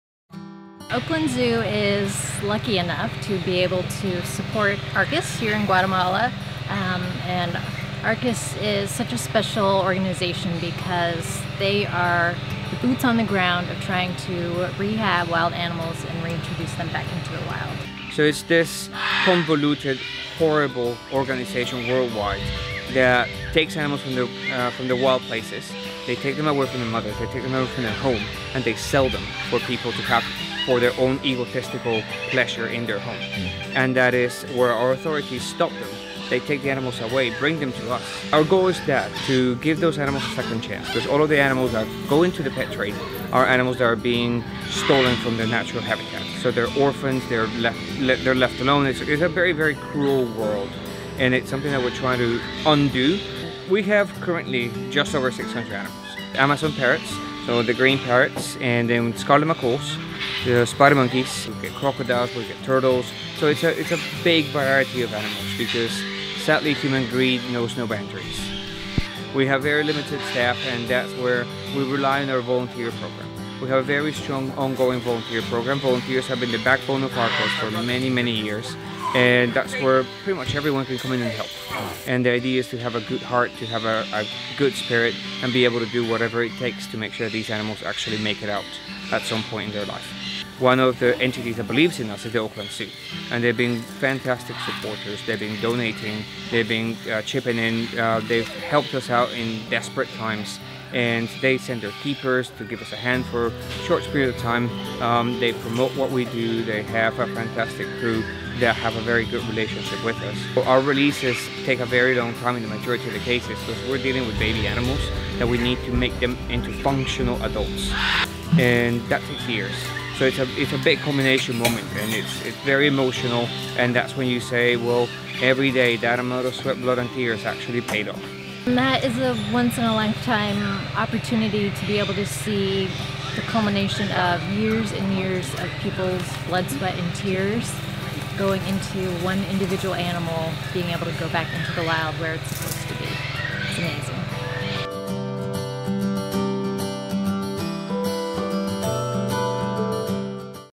ARCAS & Oakland Zoo Release Rescue Animals Back to the Wild
Oakland Zoo and the Conservation Society of California are proud partners of ARCAS in Guatemala, an amazing organization that rescues & rehabilitates animals recovered from the illegal pet trade.
Watch this video of the ARCAS team and Oakland Zoo animal care staff treating and releasing crocodiles, owls, and ocelots back into the wild!